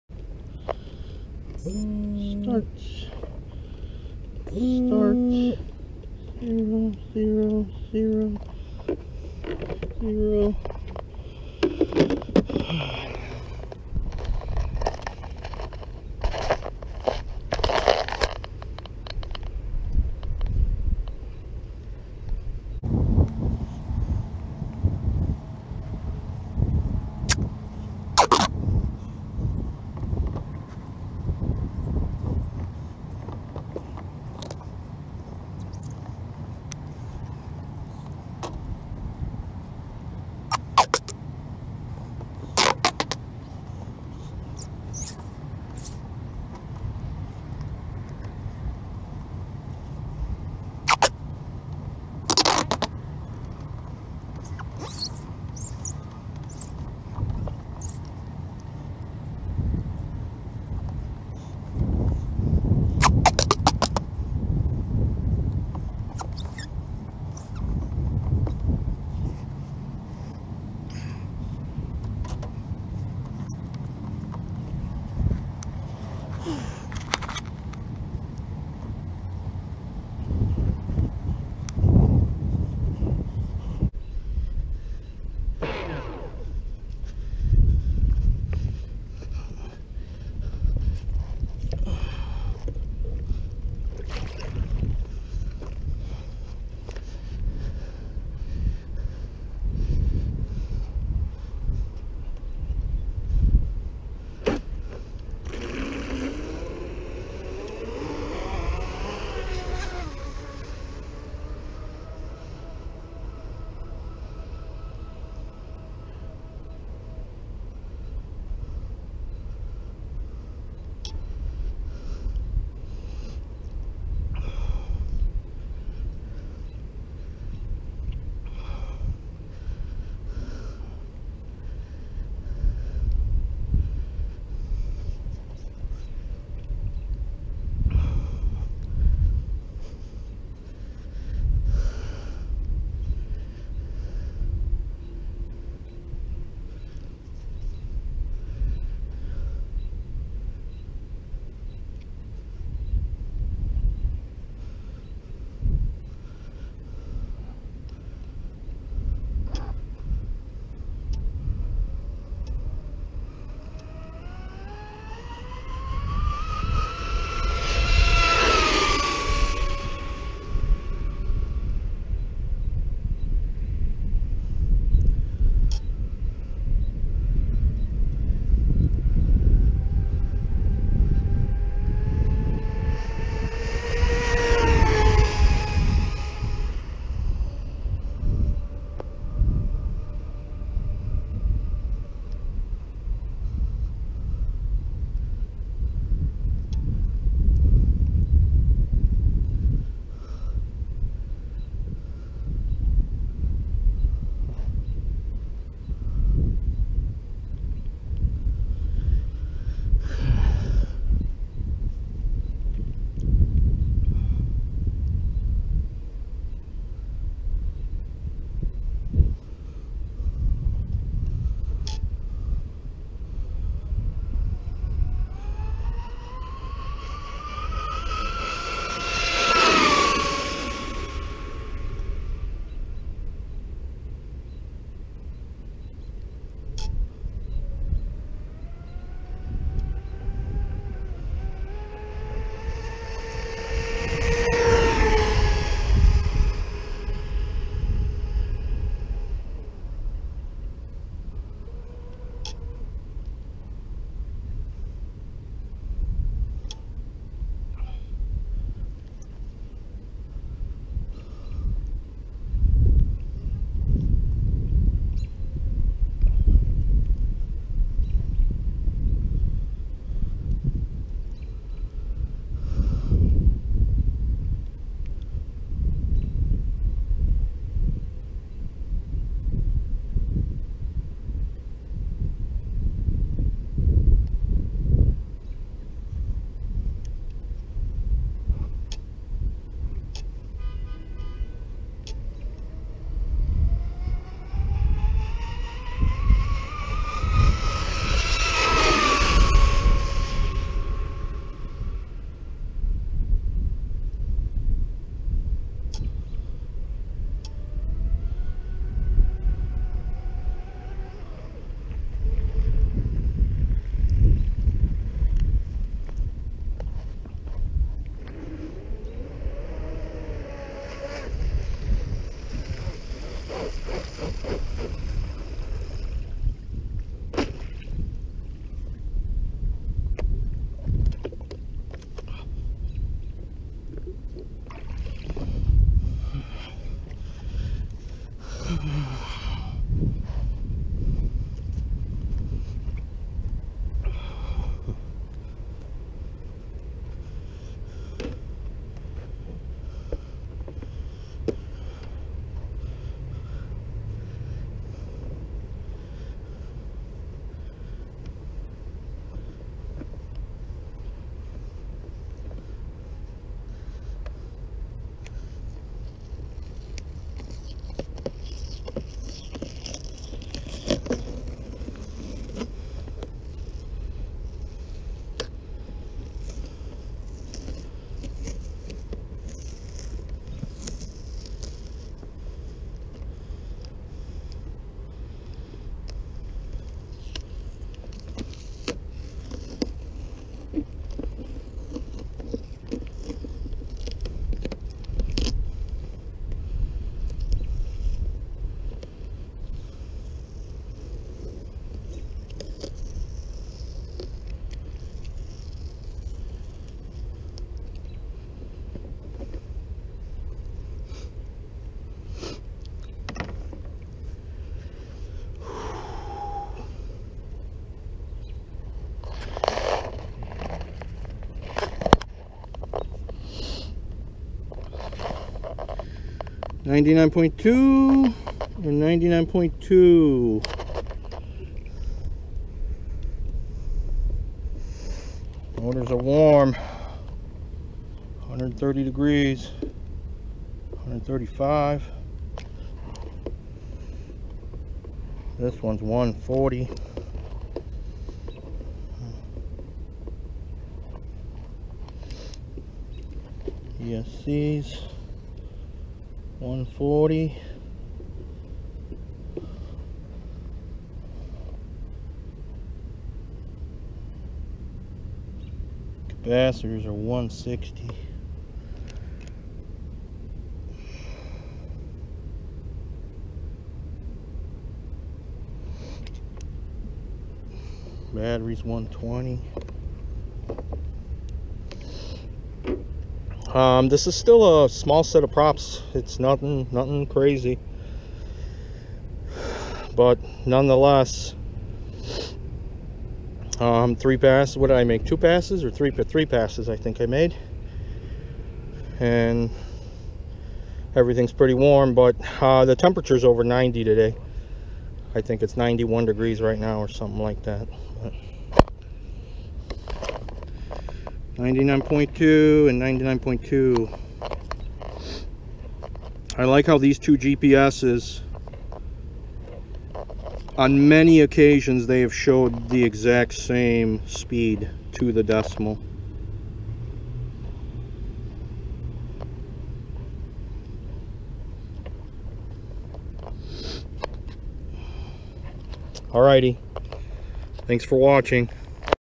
ABC 1717 prop test 4 TFL RACING ZONDA 99.2 MPH (159 KPH)
sss3674
sea king 120
6s HRB graphene 6000mah 100c
1717 prop

This prop runs even hotter then the 1716
It is not suitable for normal running or bashing
but it works great for making 2 SAW type, speed run passes
followed by a cooldown.
for normal running i would suggest a 1714 or 1715 prop.